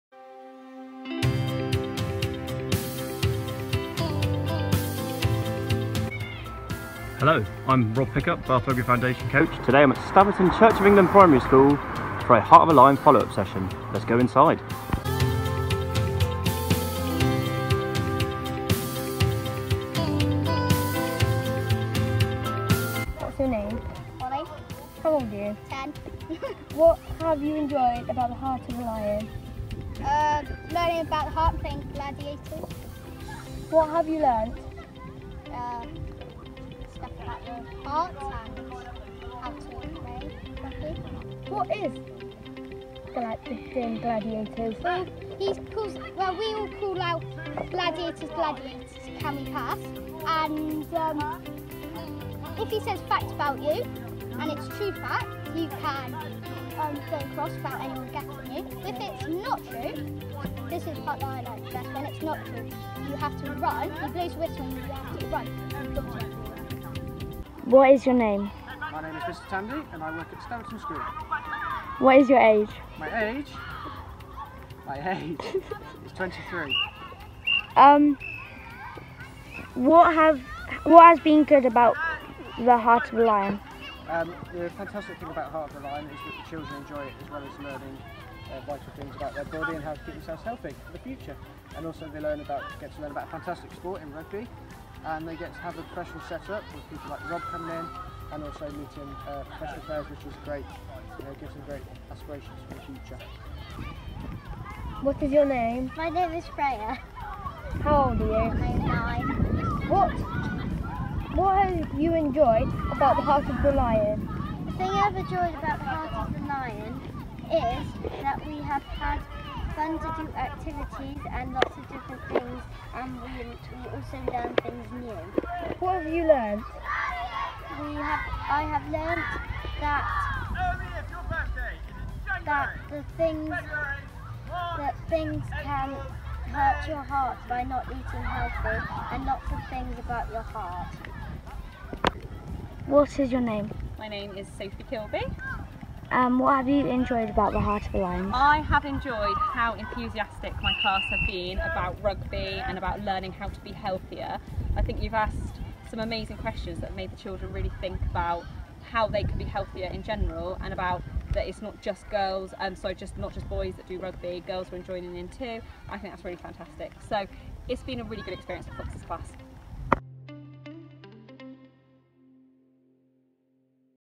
Heart of a Lion at Staverton Primary School
Heart of a Lion is an innovative health programme funded by Subway and Heart Research UK. It uses tag rugby and teaching sessions to help young people learn about how to keep their heart healthy. Students are given heart rate trackers and undertake different activities to see how it affects their heart rate.

Childhood obesity rates are at an all-time high. Today, 1 in 4 children aged 4 to 5 years old in B&NES are overweight – 1 in 10 of them are obese. And yet many young people are simply not aware of how a poor diet and not enough activity can impact their health – and their heart. This problem extends to parents too. 79% of parents with an overweight child not recognising that they are. Of those that do, 41% do not realise that it is a health risk. We know that physical activity produces overall physical, psychological and social benefits, and that inactive children are likely to become inactive adults.

This video was taken by Foundation coach Rob at Staverton Primary School who have been enjoying the Heart of a Lion programme.

Project funded thanks to a HRUK and SUBWAY® Healthy Heart Grant